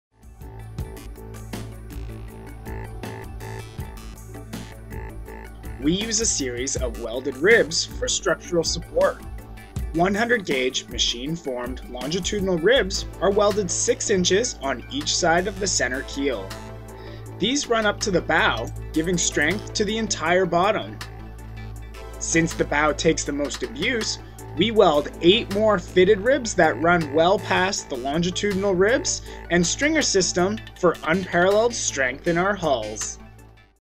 3BU Welded Ribs
Smoker Craft Inc gives you an inside look at their welded ribs!